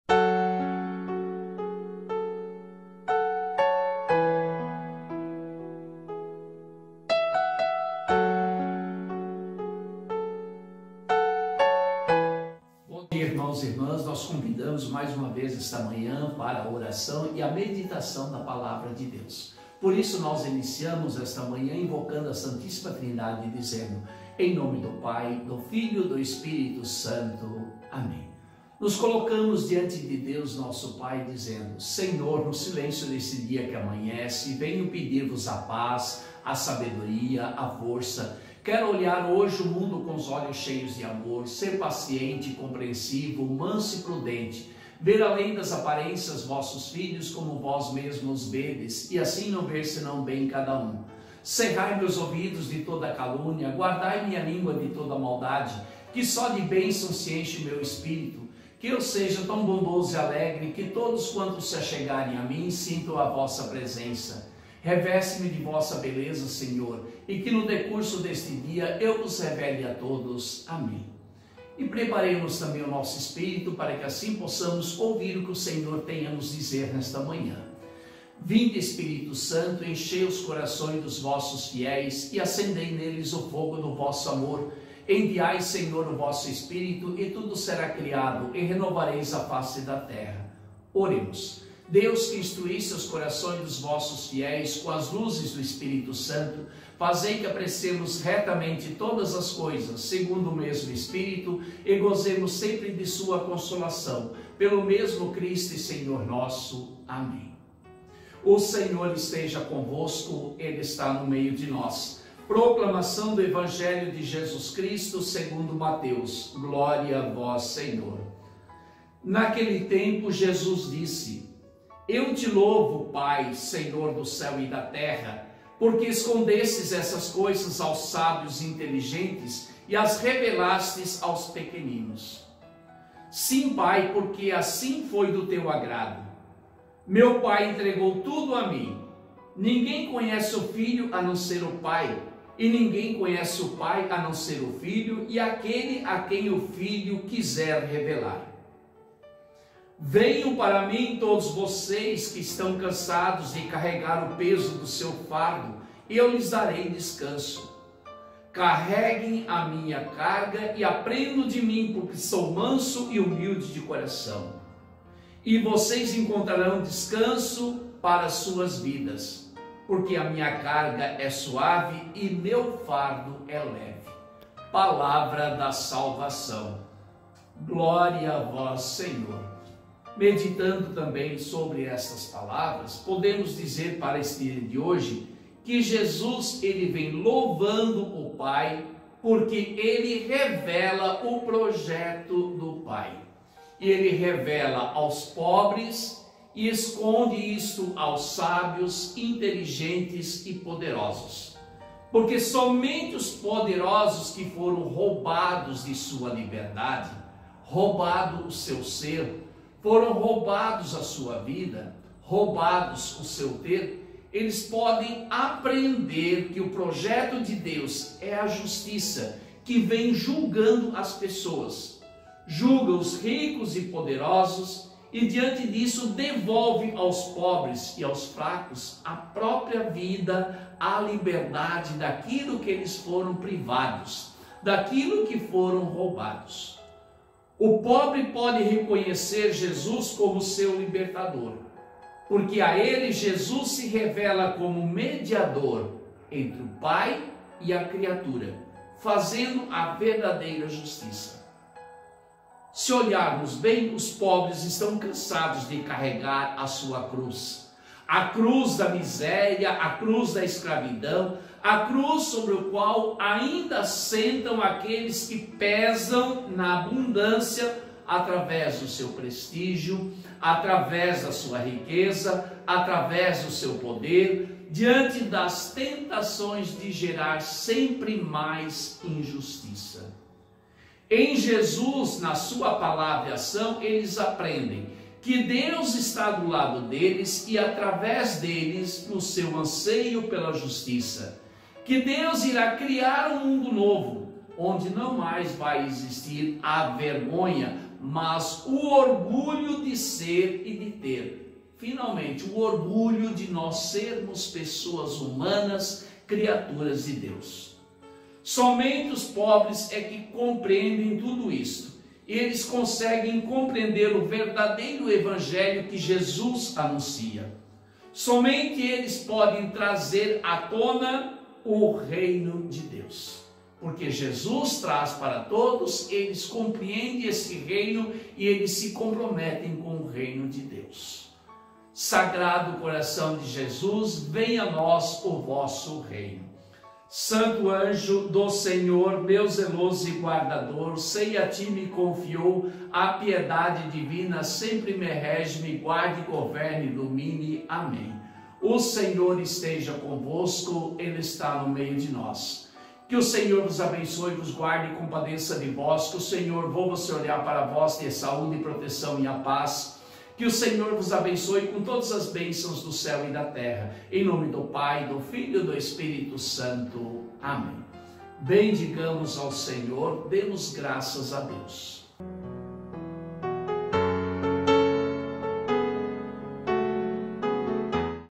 Meditação da Palavra de Deus
Meditação diária da Palavra de Deus, diretamente de nossa igreja Matriz Nossa Senhora dos Navegantes, todos os dias de segunda a sábado a partir das 06:00h da manhã, acompanhe também a partir das 18:00hs a Bênção com nosso pároco Padre Carlos Henrique.
Curta e compartilhe nosso canal de evangelização, contribua convidando parentes e amigos a se inscreverem.